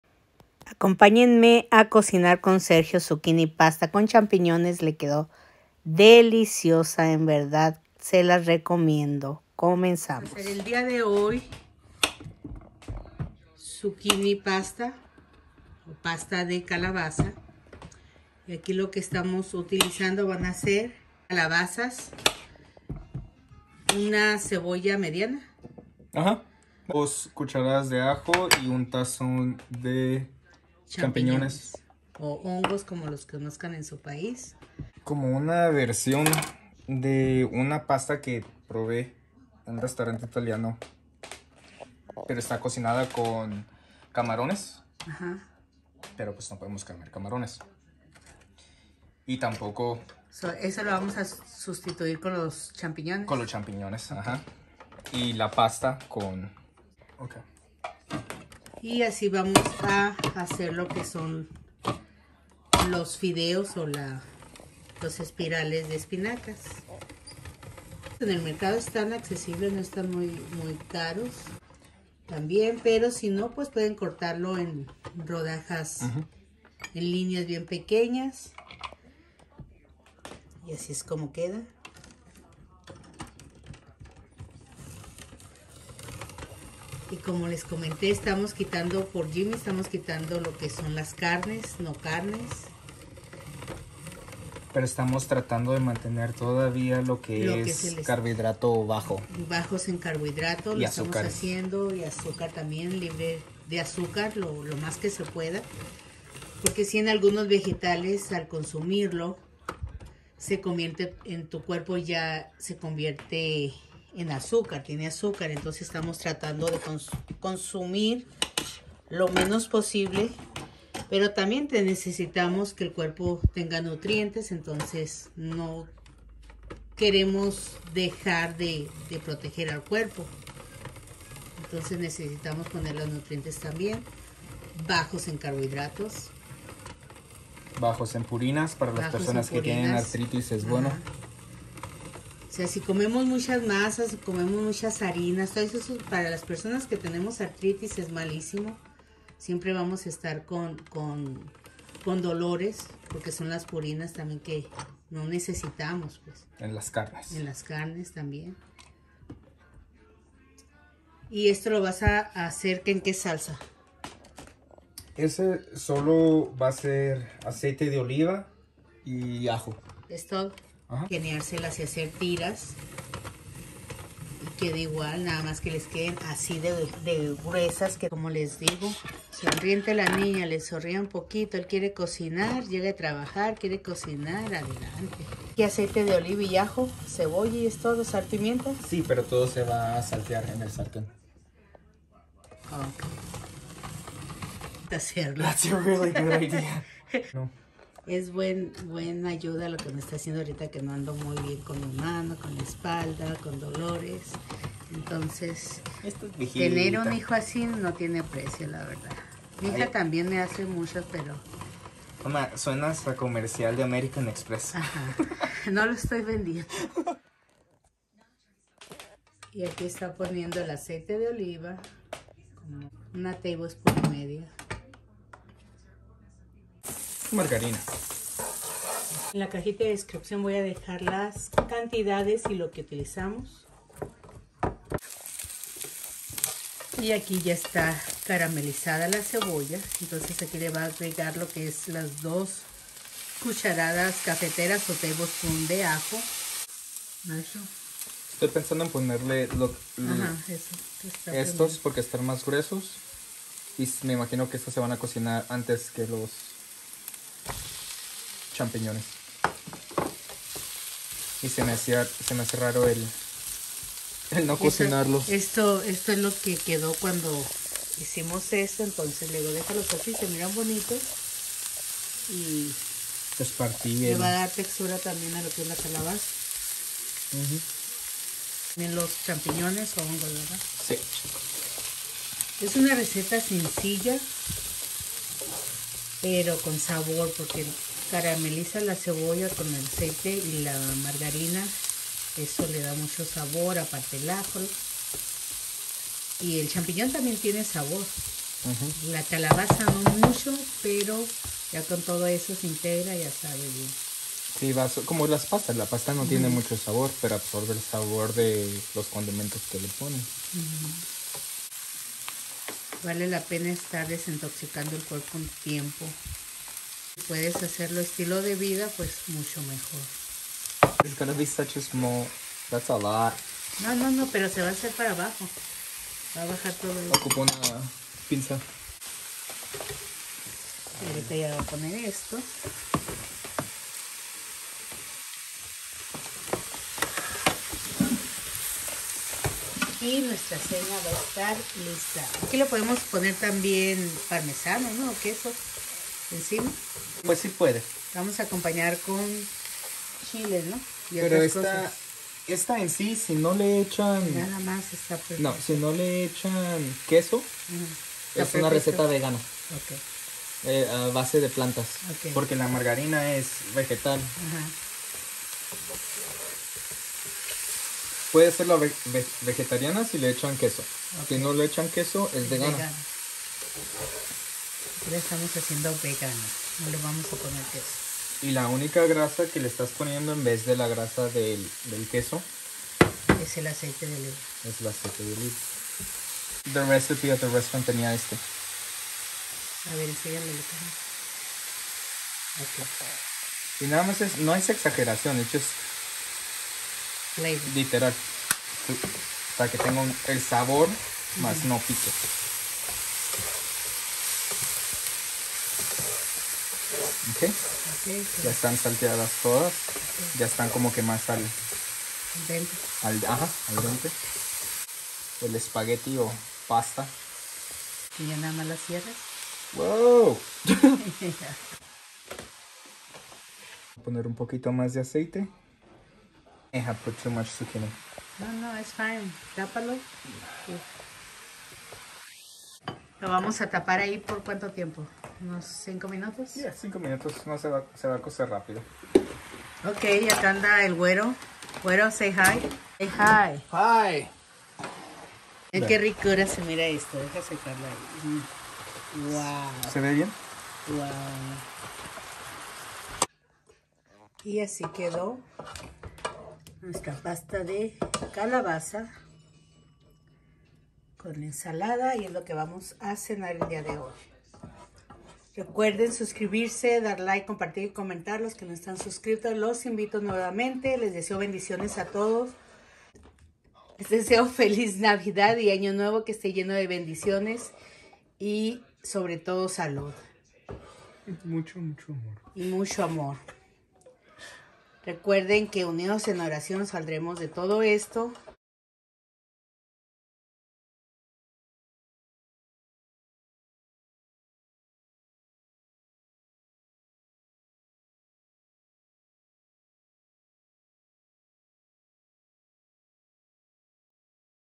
ZUCCHINI PASTA CON CHAMPIÑONES/LOW CARB
7calabazas
2tbsp de aceite de oliva
2 Tbsp de ajo molido
Sal pimienta al gusto
Cebolla chica
2tbsp de margarina
1 taza y media de champiñones